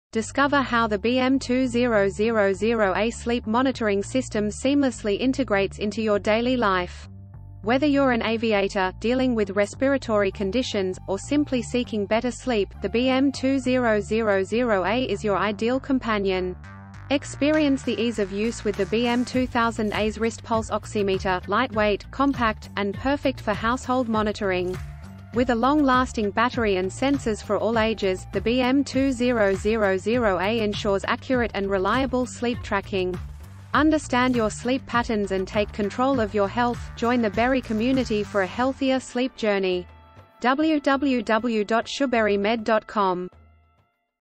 Transform Your Sleep: The BM2000A Monitoring System for Every Lifestyle
Welcome to a new era of sleep monitoring with the BM2000A Sleep Monitoring System by Berry. 🌟 Whether you're an aviator, someone managing respiratory conditions, or just looking to improve your sleep quality, the BM2000A is here to revolutionize your nightly rest.

🚀🛌 Perfect for a wide range of lifestyles, the BM2000A is not just a device; it's your nightly companion for better health. Its wrist pulse oximeter is designed for comfort and convenience, making it ideal for household monitoring.

📲 Stay ahead with the Berry Smart Health app. Seamlessly monitor your sleep patterns, analyze trends, and share your sleep reports with ease. It's sleep tracking made smarter and more connected.

🔋 With its long-lasting battery and versatile sensors suitable for all ages, the BM2000A delivers accurate and reliable sleep tracking, night after night.

💡 Knowledge is power, especially when it comes to your health. Understand your sleep patterns in-depth and take proactive steps towards a healthier lifestyle.

Join the Berry community and embark on a journey towards healthier sleep. For more information and to become part of our sleep revolution,